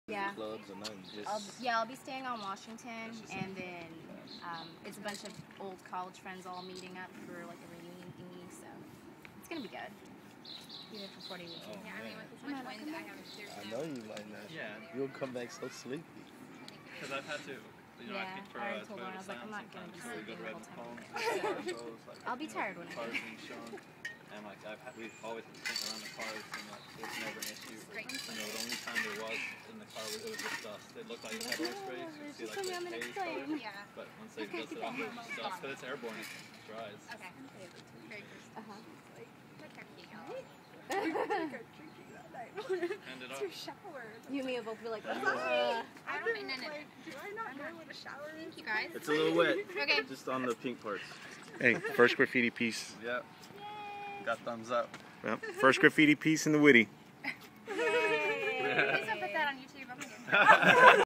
Historic Day at Witte Museum
A historic day for the graffiti culture in San Antonio. DMISE & ENKS did the first graffiti artistic piece in the Witte Museum. They will be showcased at Sneaker Palooza and the piece they create will be raffled with proceeds benefiting Big Brothers Big Sisters. You can see them live Sunday March 14th at Sneaker Palooza 6-11pm